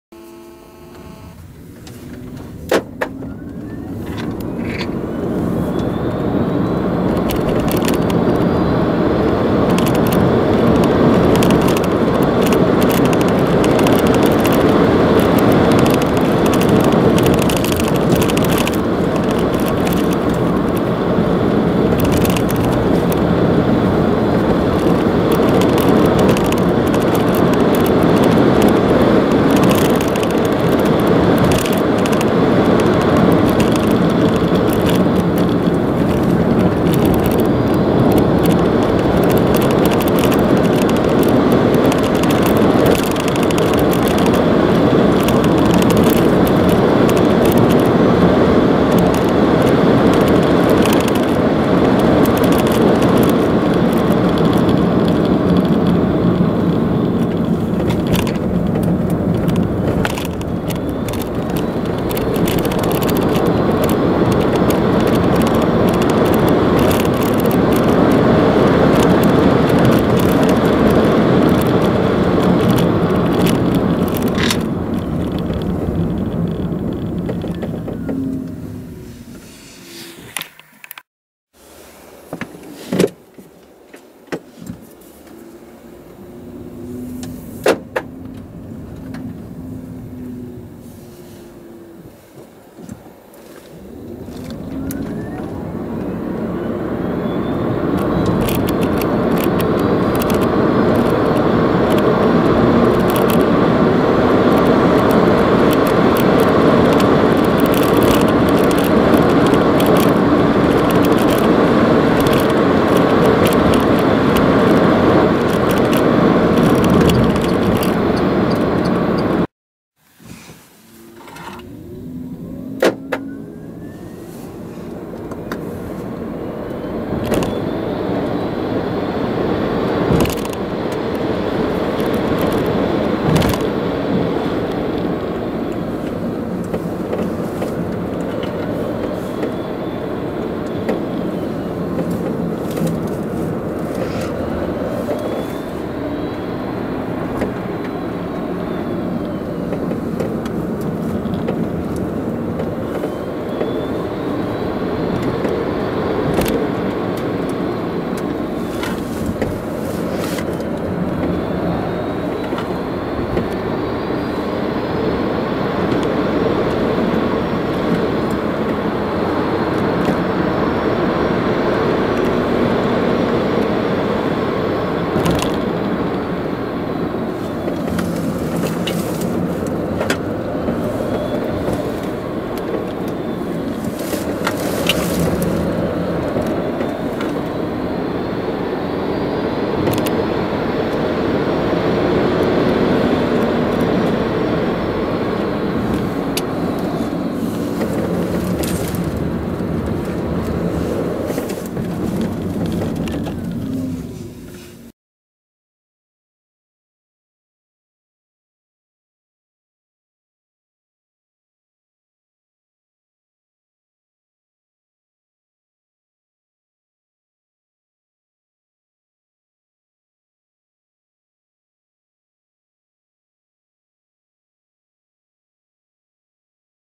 Renault ZOE Fast Driving
It drives pretty well but the ESP can't be turned off, so it slows down in the corners when you want to drive fast.

Electric 88 hp, 220 Nm
0-100 km/h: 13,8 sec
Topspeed: 135 km/h (Limited at 139 km/h)

@ Weert, The Netherlands